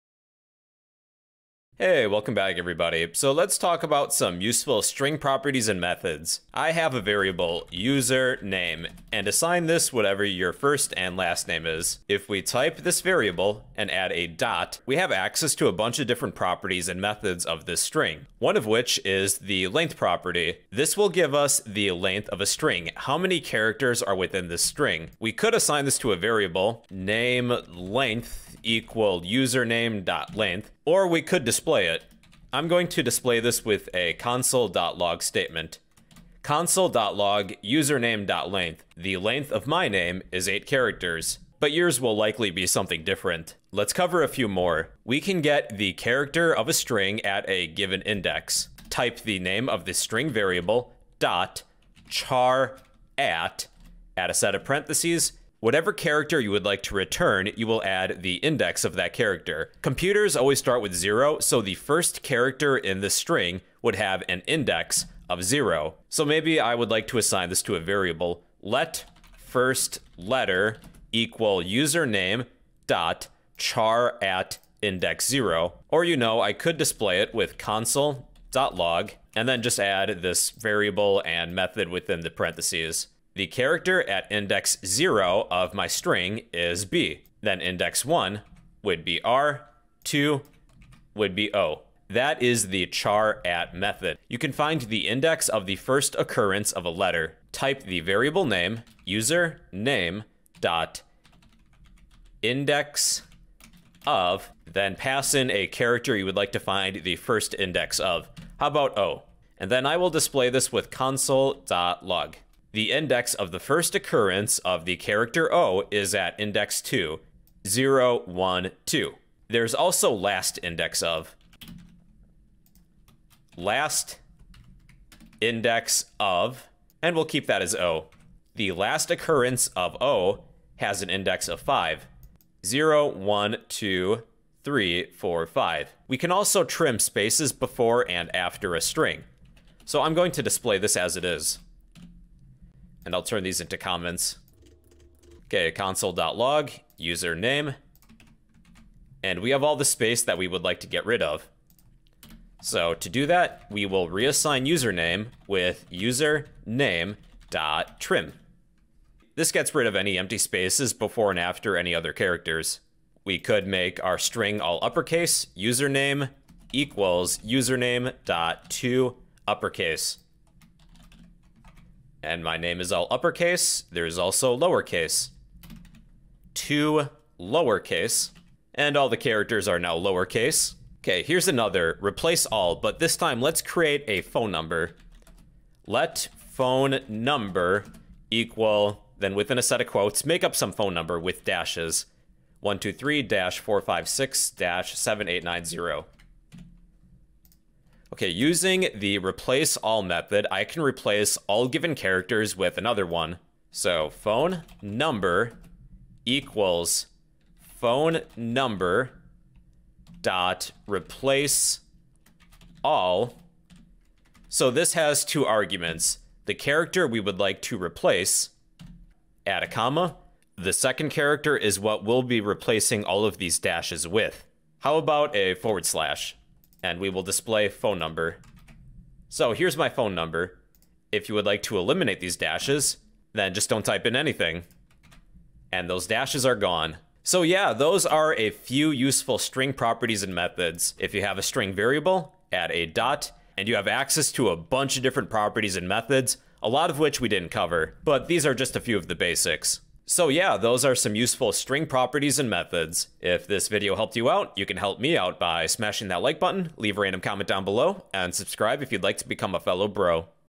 Useful string methods in JavaScript 〰️【4 minutes】
// useful string properties & methods

//console.log(userName.length);
//console.log(userName.charAt(0));
//console.log(userName.indexOf("o"));
//console.log(userName.lastIndexOf("o"));
//userName = userName.trim();
//userName = userName.toUpperCase();
//userName = userName.toLowerCase();

phoneNumber = phoneNumber.replaceAll("-", "");

console.log(phoneNumber);